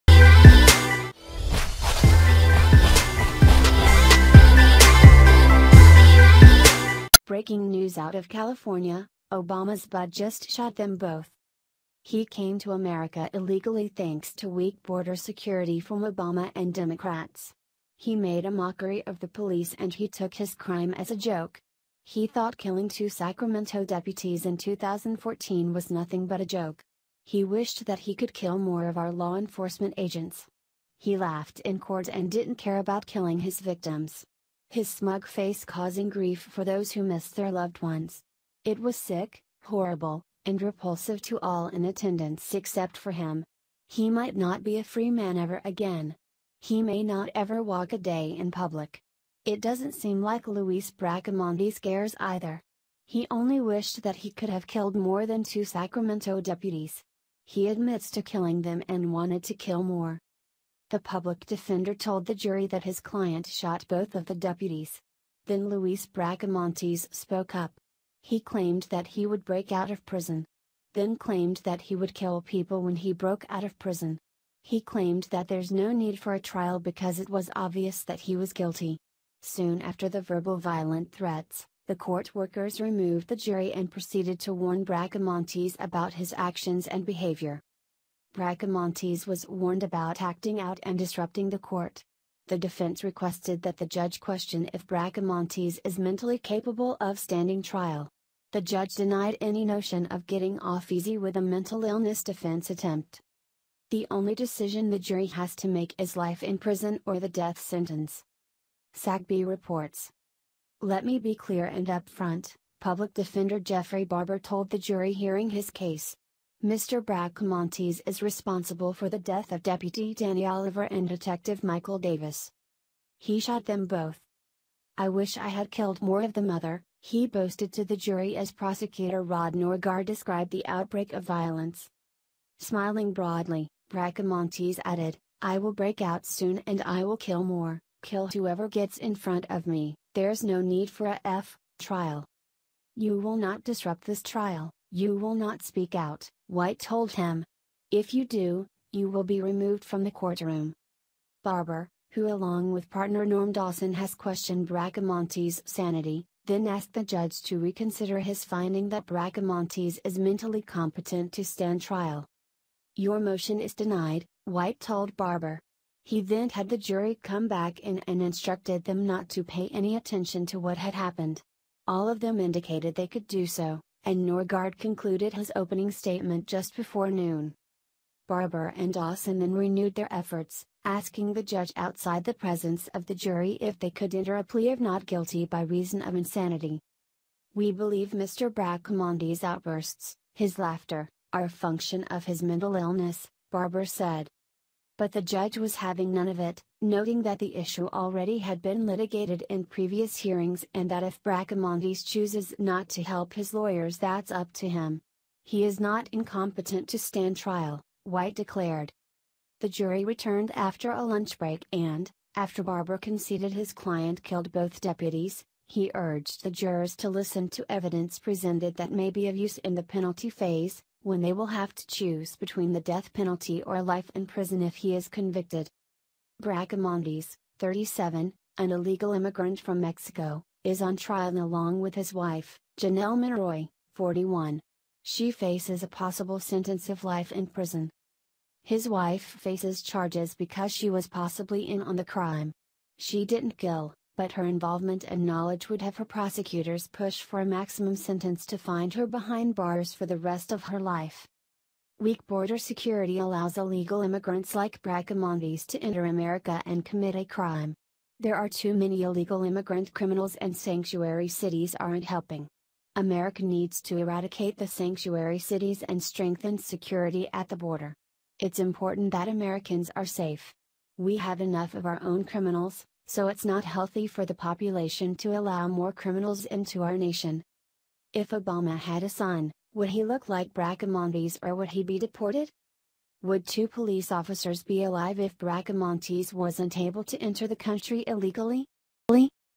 BREAKING News Out Of California – Obama’s Bud Just Shot Them BOTH!
He came to America illegally thanks to weak border security from Obama and Democrats. He made a mockery of the police and he took his crime as a joke. He thought killing two Sacramento deputies in 2014 was nothing but a joke. He wished that he could kill more of our law enforcement agents. He laughed in court and didn’t care about killing his victims. His smug face causing grief for those who missed their loved ones. It was sick, horrible, and repulsive to all in attendance except for him. He might not be a free man ever again. He may not ever walk a day in public. It doesn’t seem like Luis Bracamontes cares either. He only wished that he could have killed more than two Sacramento deputies. He admits to killing them and wanted to kill more.

The public defender told the jury that his client shot both of the deputies. Then Luis Bracamontes spoke up. He claimed that he would break out of prison. Then claimed that he would kill people when he broke out of prison. He claimed that there’s no need for a trial because it was obvious that he was guilty. Soon after the verbal violent threats, the court workers removed the jury and proceeded to warn Bracamontes about his actions and behavior.

Bracamontes was warned about acting out and disrupting the court. The defense requested that the judge question if Bracamontes is mentally capable of standing trial. The judge denied any notion of getting off easy with a mental illness defense attempt.

The only decision the jury has to make is life in prison or the death sentence.
SacBee reports:
“Let me be clear and up front,” public defender Jeffrey Barbour told the jury hearing his case. “Mr. Bracamontes is responsible for the death of Deputy Danny Oliver and Detective Michael Davis.
“He shot them both.”

“I wish I had killed more of the mother——-,” he boasted to the jury as prosecutor Rod Norgaard described the outbreak of violence.

Smiling broadly, Bracamontes added, “I will break out soon and I will kill more, kill whoever gets in front of me … There’s no need for a f—— trial.”

“You will not disrupt this trial, you will not speak out,” White told him. “If you do, you will be removed from the courtroom.”

Barbour, who along with partner Norm Dawson has questioned Bracamontes’ sanity, then asked the judge to reconsider his finding that Bracamontes is mentally competent to stand trial.

“Your motion is denied,” White told Barbour. He then had the jury come back in and instructed them not to pay any attention to what had happened. All of them indicated they could do so, and Norgaard concluded his opening statement just before noon.

Barbour and Dawson then renewed their efforts, asking the judge outside the presence of the jury if they could enter a plea of not guilty by reason of insanity.

“We believe Mr. Bracamontes’ outbursts, his laughter, are a function of his mental illness,” Barbour said.

But the judge was having none of it, noting that the issue already had been litigated in previous hearings and that if Bracamontes chooses not to help his lawyers that’s up to him. “He is not incompetent to stand trial,” White declared.

The jury returned after a lunch break and, after Barbour conceded his client killed both deputies, he urged the jurors to listen to evidence presented that may be of use in the penalty phase, when they will have to choose between the death penalty or life in prison if he is convicted.

Bracamontes, 37, an illegal immigrant from Mexico, is on trial along with his wife, Janelle Monroy, 41. She faces a possible sentence of life in prison.”

His wife faces charges because she was possibly in on the crime. She didn’t kill, but her involvement and knowledge would have her prosecutors push for a maximum sentence to find her behind bars for the rest of her life.

Weak border security allows illegal immigrants like Bracamontes to enter America and commit a crime. There are too many illegal immigrant criminals and sanctuary cities aren’t helping. America needs to eradicate the sanctuary cities and strengthen security at the border. It’s important that Americans are safe. We have enough of our own criminals, so it’s not healthy for the population to allow more criminals into our nation.

If Obama had a son, would he look like Bracamontes or would he be deported?

Would two police officers be alive if Bracamontes wasn’t able to enter the country illegally?